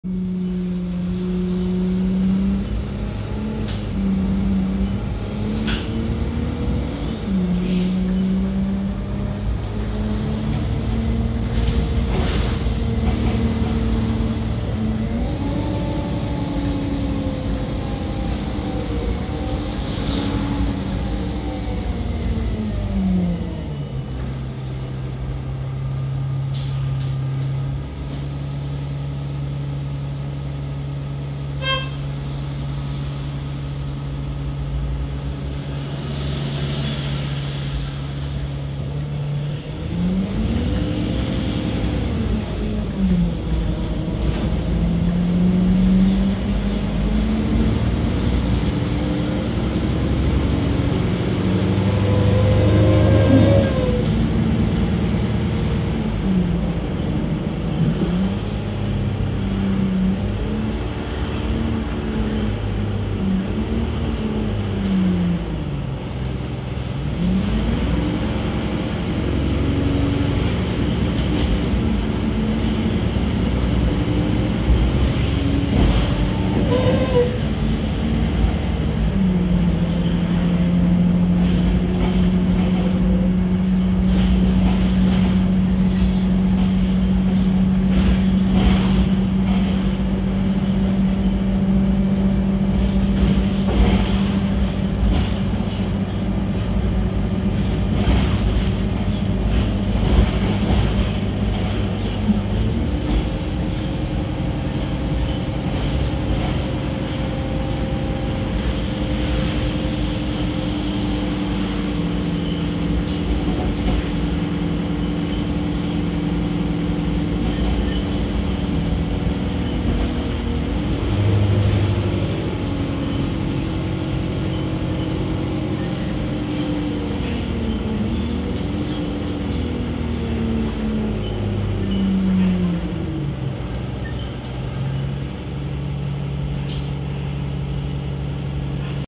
Squeaky brakes! Bee Network - Stagecoach Manchester 183 service between Oldham and Oldham Hospital
Bee Network - Stagecoach Manchester 183 service between Limeside and the Royal Oldham Hospital, filmed between Oldham Bus Station and the Royal Oldham Hospital.

27949 - SN13 CLF (ex First 67420)
ADL E300